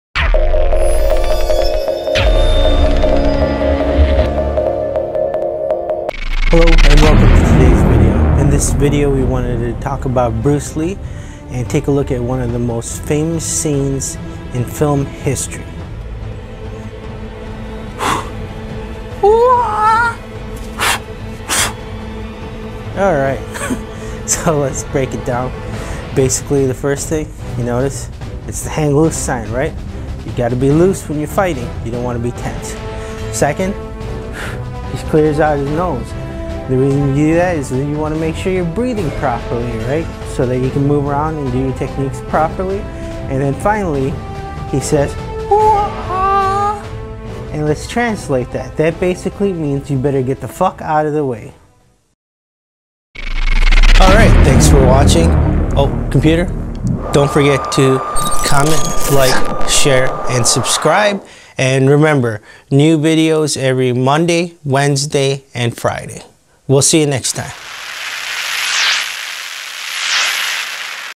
Decoding Bruce Lee
Decoding Bruce Lee. In this video we will take a look at what Bruce Lee meant with his body language. Bruce Lee was a true legend and the father of MMA. Please note this is Unlock Your Journey's interpretation. Did you know Bruce Lee is now a playable  character in the UFC game?